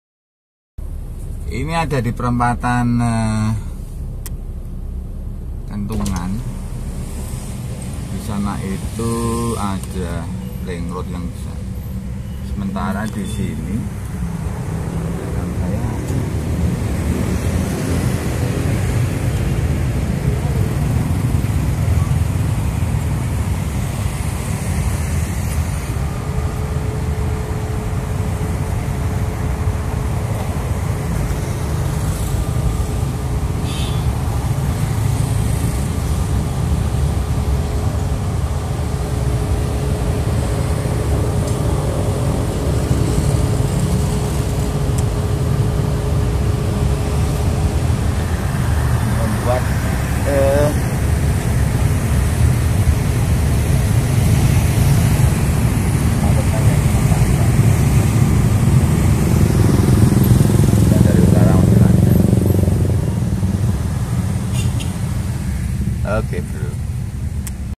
Parah..!!! Bus Trans Jogja mogok di Perempatan Kentungan
Menyebabkan kemacetan panjang